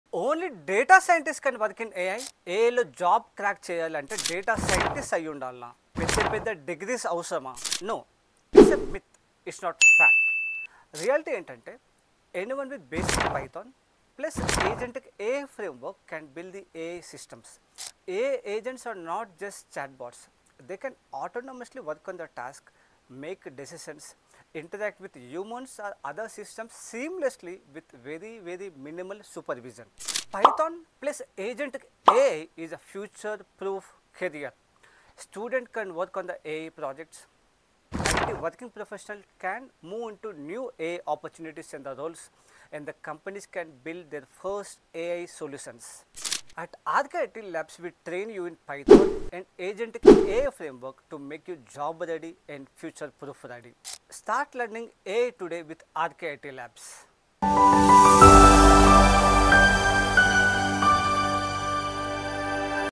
Agentic AI: Myth Busted: Only Data Scientists Work in AI? || Rkitlabs
Agentic AI: Myth Busted: Only Data Scientists Work in AI?
Agentic AI is the future of coding, and guess what? You do not need a Data Science PhD to build it!

That's right, the biggest tech myth is officially busted!
You don't need years of specialized study to build powerful, cutting-edge AI systems.
The reality is simple: Basic Python + Agentic AI frameworks (like LangChain, CrewAI, RAG) is the new skill stack that launches careers.

In this quick reel, we break down how Agentic AI is revolutionizing automation and exactly what skills you actually need to become an 'AI Builder' (creating Intelligent Systems & Automation Agents).

Future-proof your career: Learn how to build Agentic AI Agents that can autonomously plan, make decisions, and execute tasks. This is the next massive shift for coders and career changers!

Don't let outdated myths hold your career back.

Address: Portland Oregon, USA

Ceo & Founder: Ravindar Kothapally